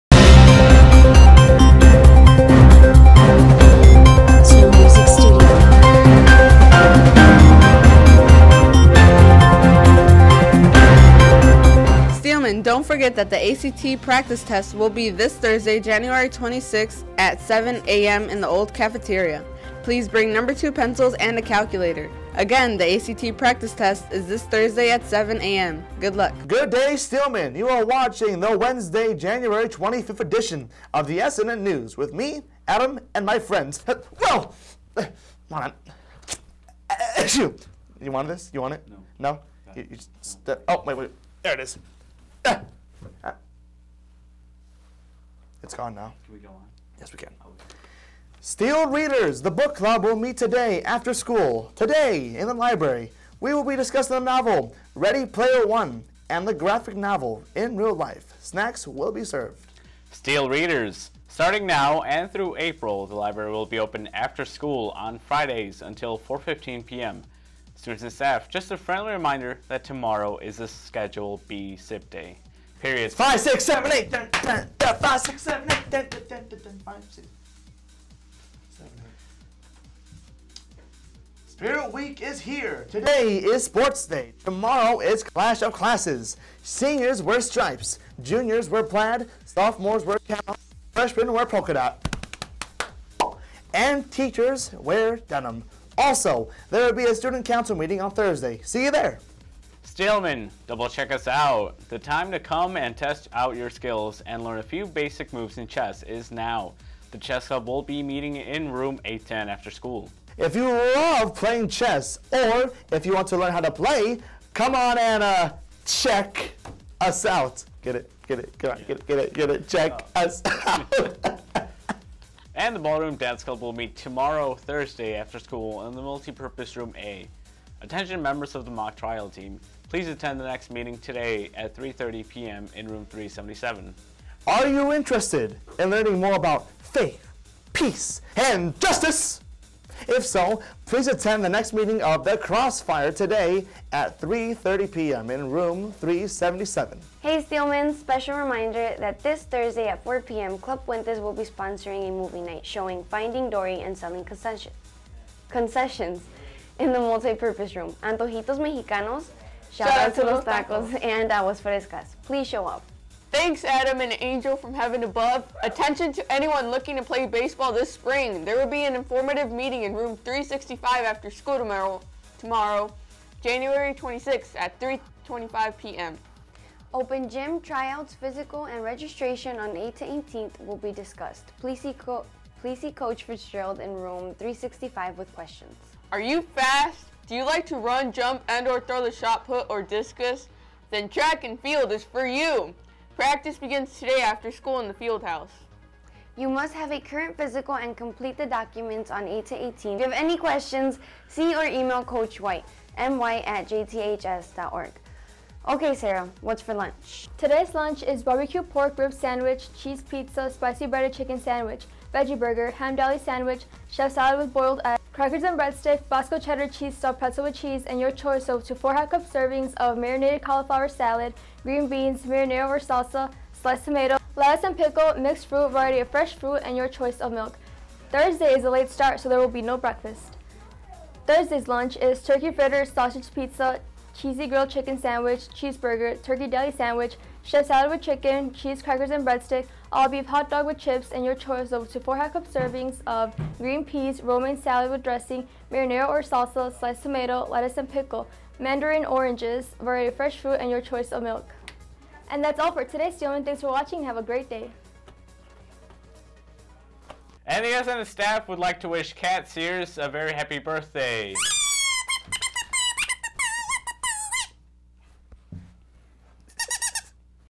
WEDNESDAY JANUARY 25, 2017
THE SNN NEWS WAS RECORDED AT JOLIET CENTRAL'S TV STUDIO UNDER THE DIRECTION OF MR. DON GRAY